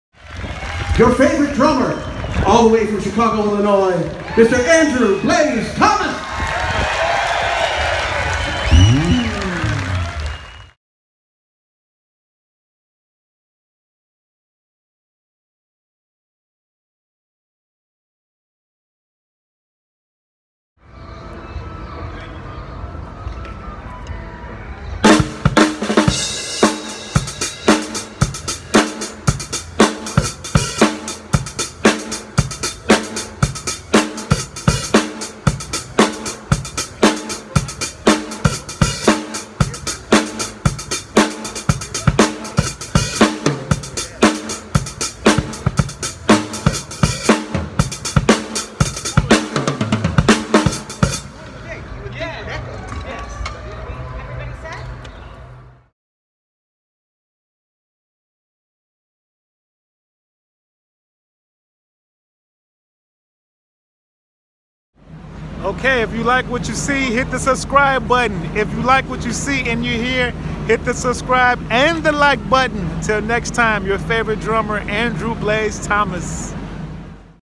Big Gig Warm Up
Warming up on the drums at Chicago Blues fest, the biggest outdoor Blues fest in the world.